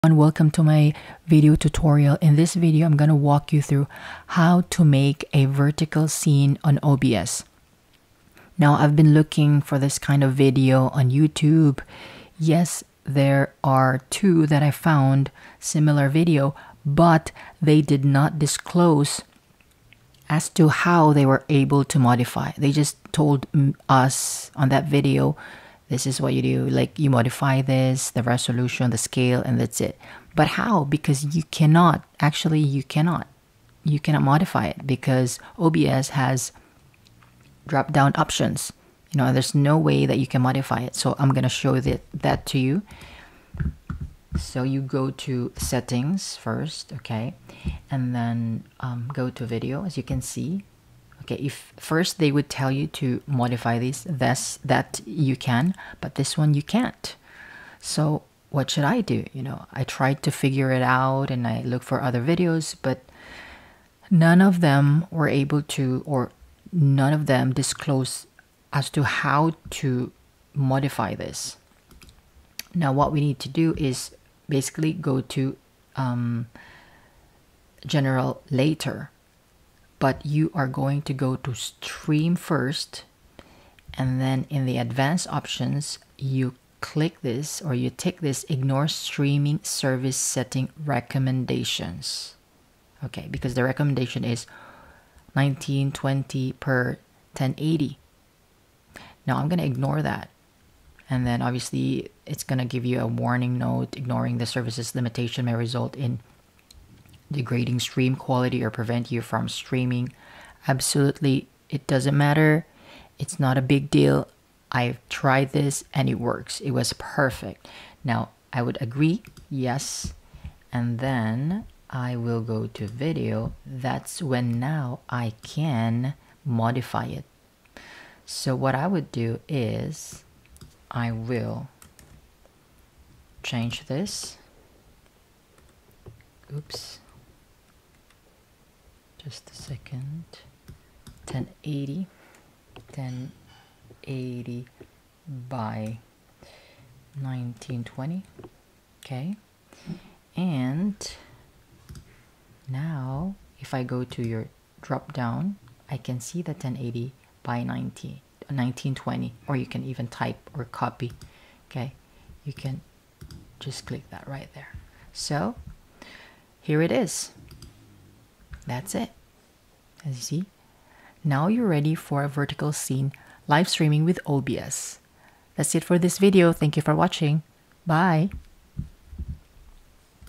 How to Stream Vertical in OBS for Facebook and YouTube Scene: Quick Guide in 2025
How to Stream Vertical Scene in OBS?
Are you ready to take your live streaming to the next level? In this comprehensive tutorial, we'll show you how to set up vertical streaming in OBS (Open Broadcaster Software) for both Facebook and YouTube. Whether you're a content creator, gamer, or just want to share your moments in a vertical format, this step-by-step guide will help you configure your settings, optimize your stream, and engage your audience like never before.